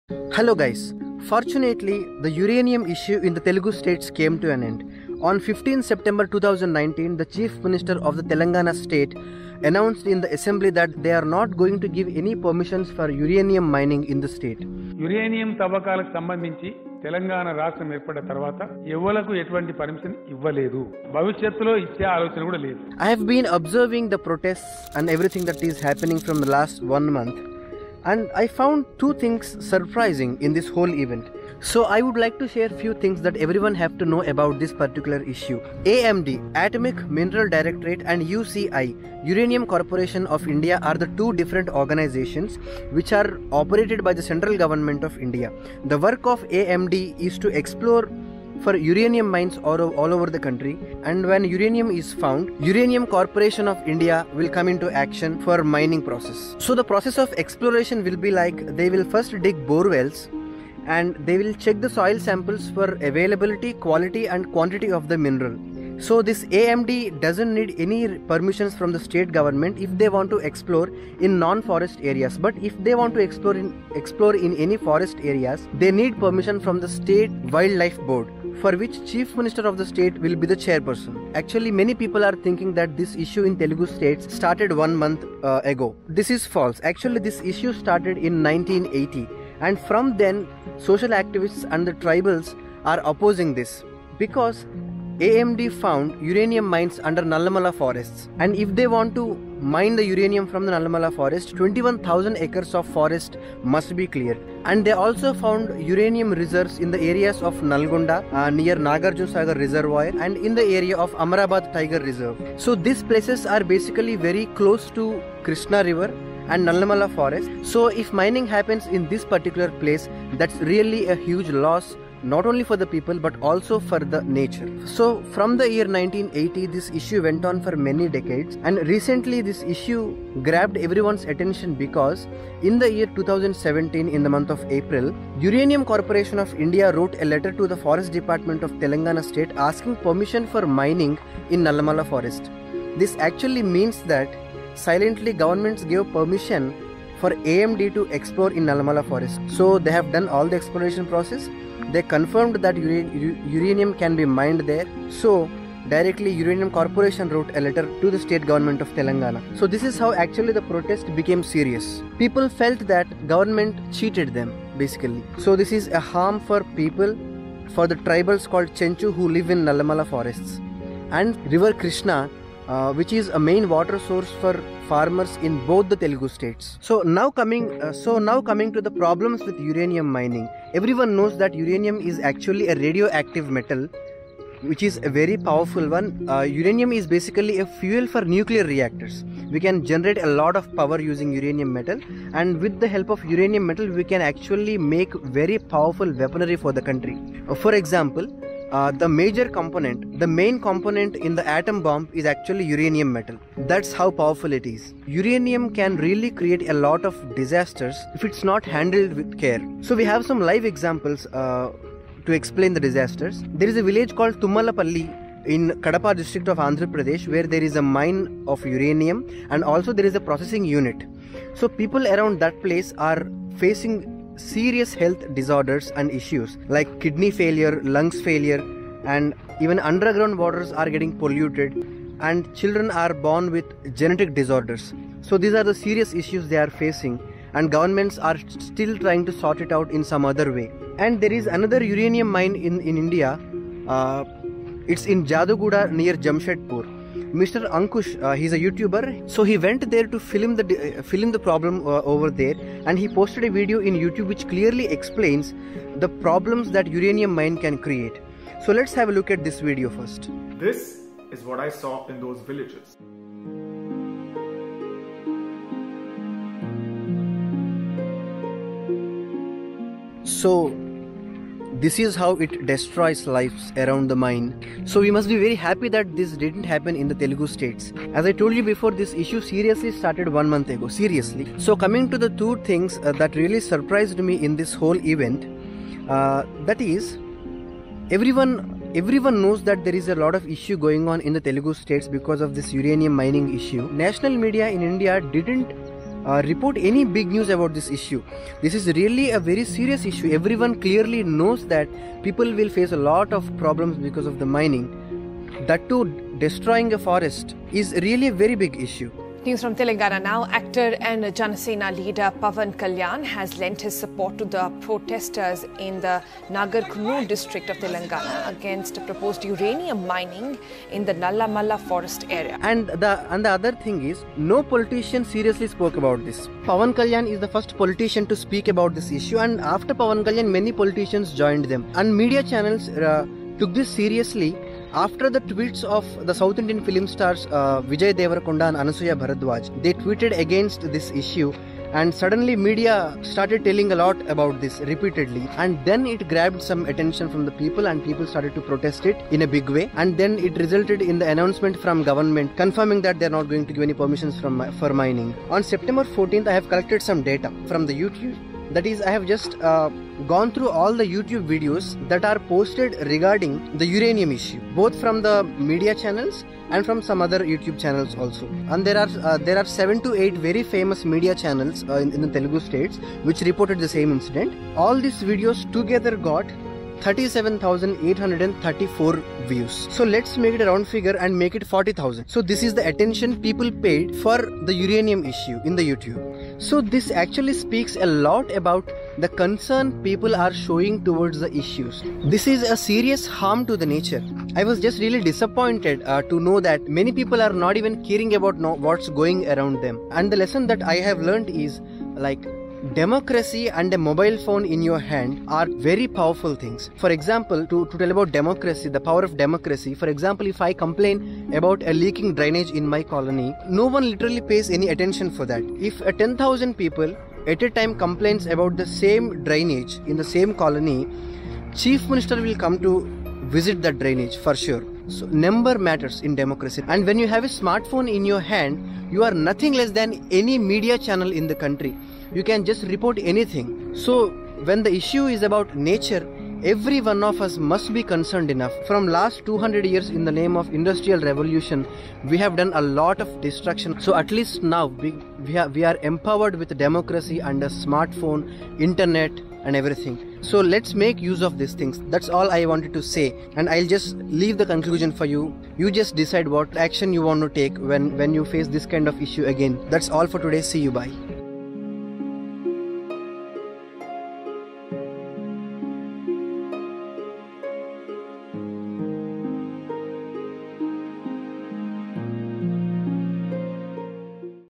Uranium Mining In Nallamala Forest | EXPLAINED!
Narrator : Aditya

Uranium mining is very harmful to the environment and the living beings. Recently, there were many protests against uranium mining plans in Nallamala forest which affects lives and environment in Telangana and Andhra Pradesh states. In this video, we've explained everything about it, clearly. This video also shows examples of how it affected peoples' lives in India previously due to Uranium mining in Jadugoda area. Uranium is radioactive and it is very harmful to our lives and the environment. Radioactivity threatens human existence!

Pawan Kalyan was the first politician to speak up against uranium mining. Because of various protests against the uranium mining, it then grabbed attention in the media and in public, in support of a good cause to save earth from impacts that may occur in future due to uranium mining!

Let's ensure there is no other nuclear grave in India!